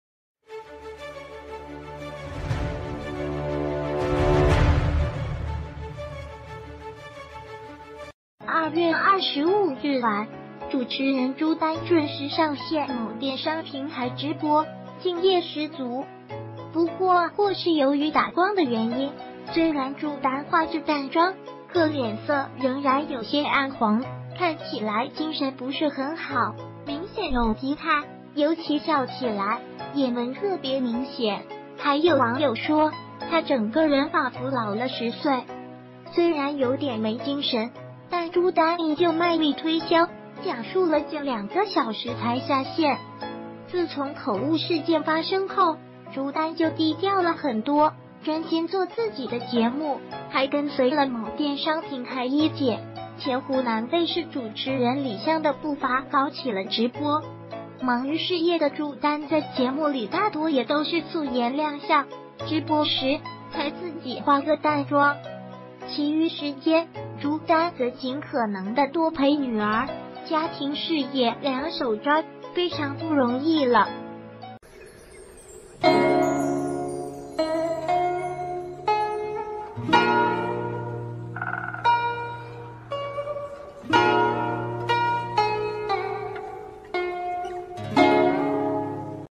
朱丹也跟隨李湘開直播？面色憔悴整個人似老10歲
2月25日晚，主持人...